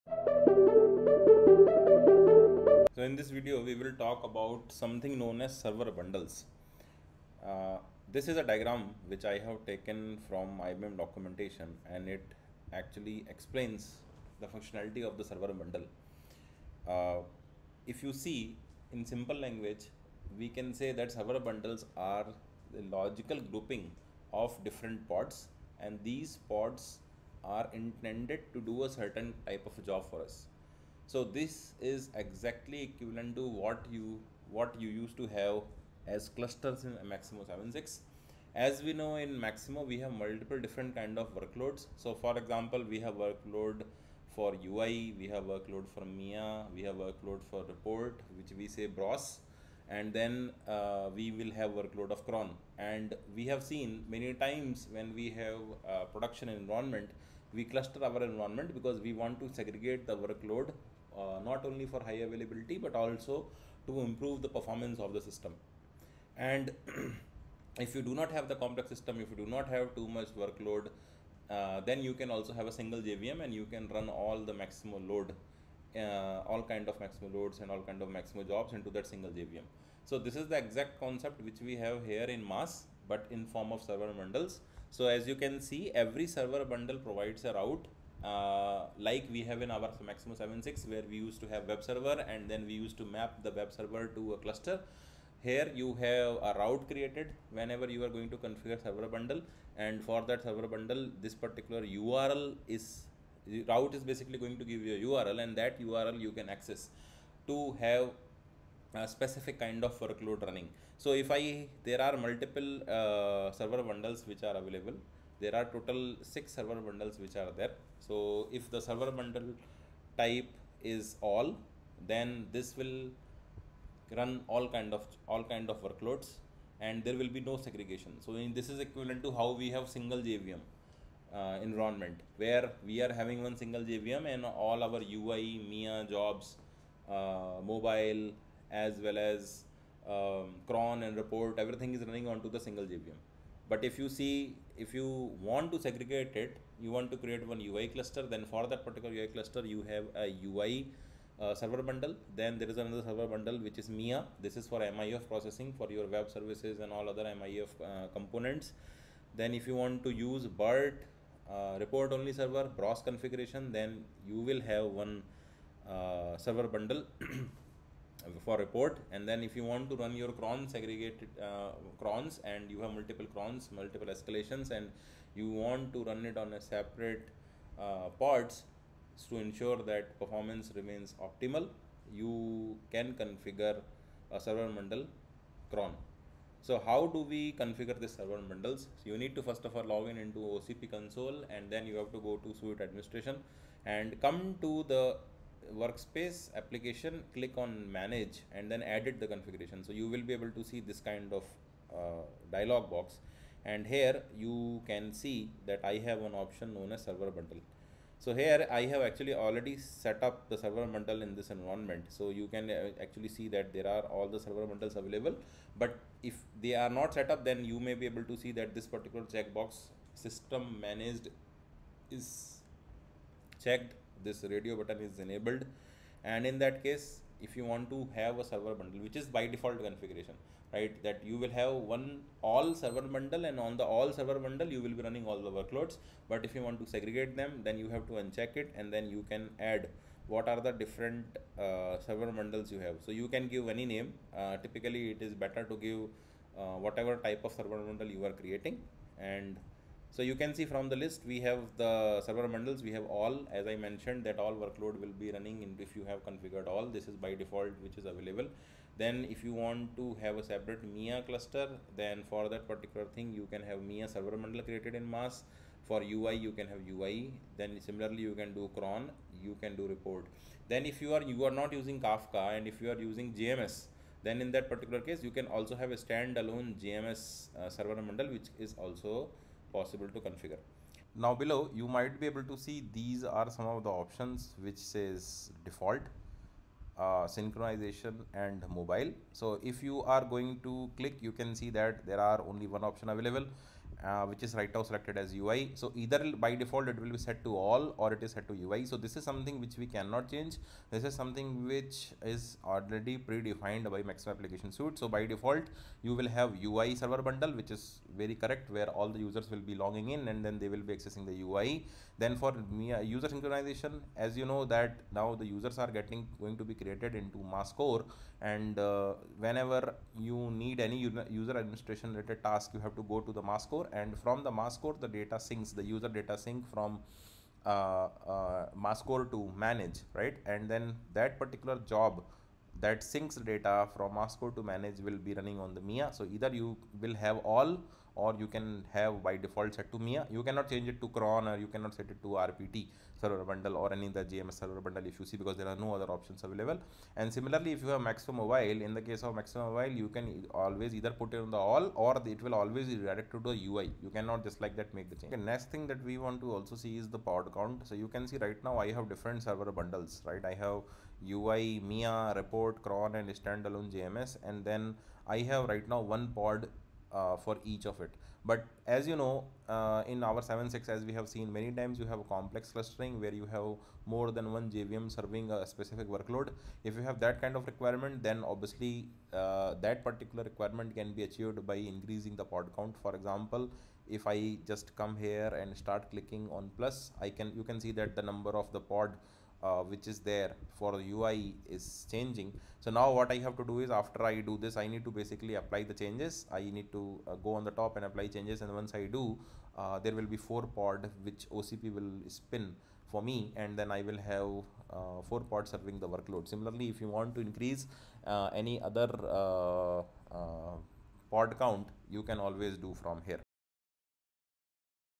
Let's Learn MAS | EP09 - How to setup Clustering in MAS - Server Bundles
Maximo Application Suite (MAS 9) runs on IBM Redhat Openshift also known as OCP. In this video we talk about what changes have happened in MAS Clustering and how we can have similar setup like Maximo 76 clustering in MAS